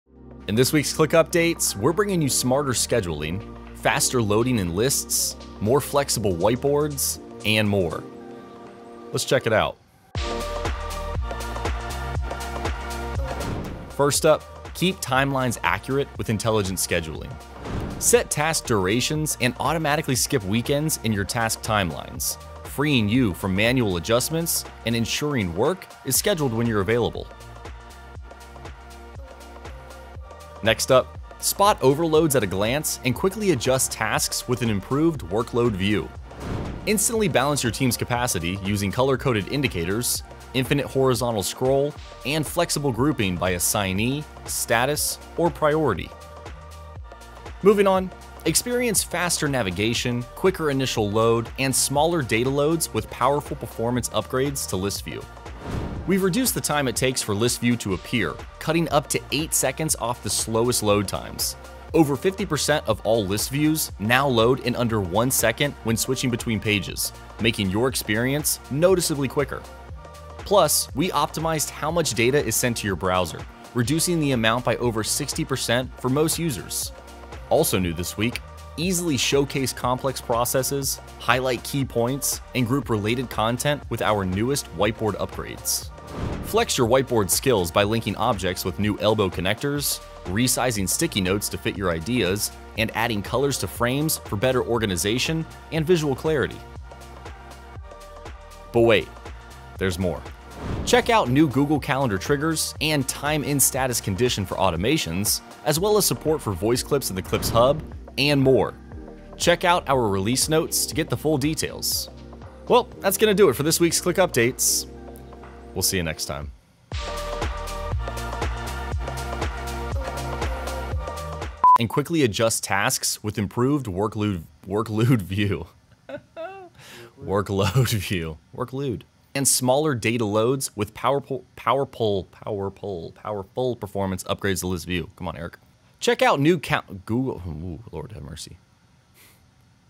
ClickUp Release 3.58
This week's release brings smarter scheduling, faster loading in Lists, more flexible Whiteboards—and much more!

Chapters:
0:00 Intro
0:14 Intelligent Scheduling
0:31 Improved Workload View
0:48 Upgraded List View
1:17 Whiteboard Upgrades
1:39 And More!